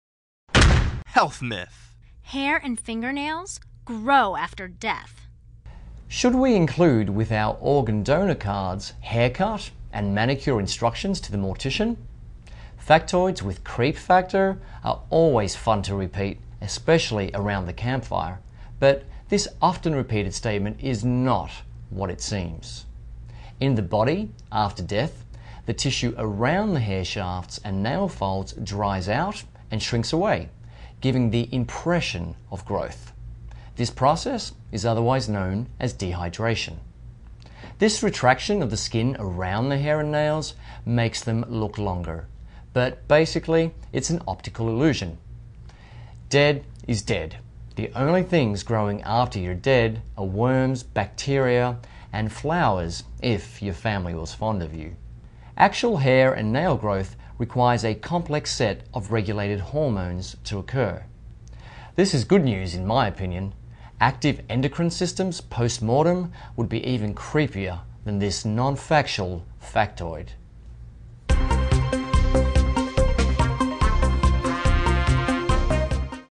Do Hair And Fingernails Grow After Death? (Health Guru)
Should we include with our organ-donor cards, haircut and manicure instructions to the mortician? Watch this!